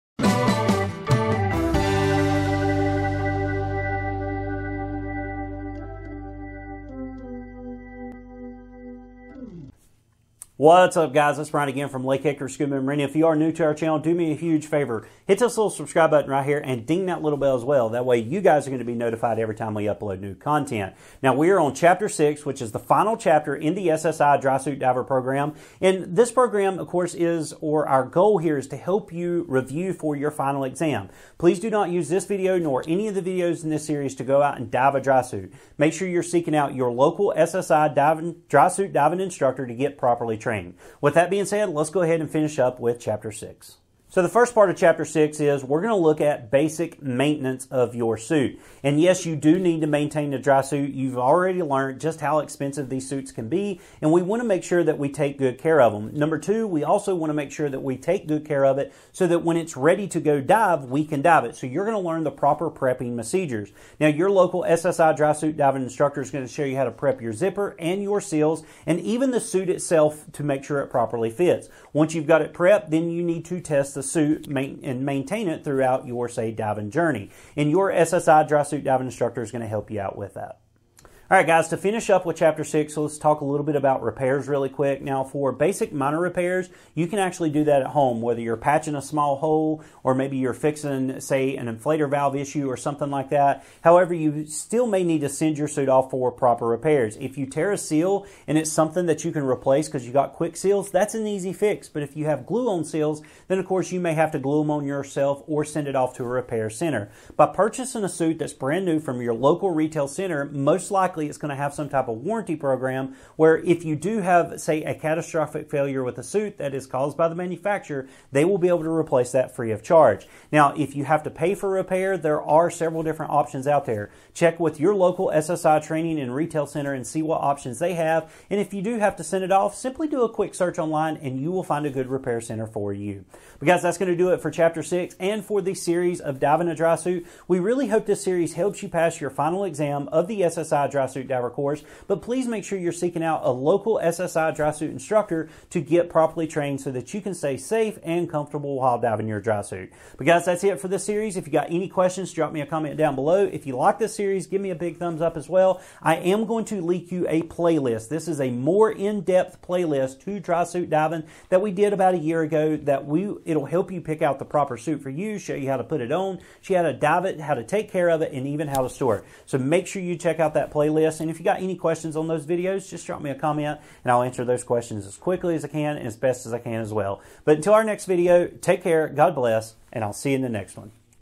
SSI Dry Suit Diver Chapter 6 Review
In this new video series, Instructor Trainer Bryan Stafford reviews Chapter 6 of the SSI Dry Suit Diver Program. Please do not use this video, nor any of the videos in this series to go out and dive with a Dry Suit. Make sure you are receiving proper training from your local SSI Dry Suit Instructor.

0:00 Opening
0:09 Intro
0:44 Maintenance
1:21 Repairs
2:13 Closing

Playsee LakeHickoryScuba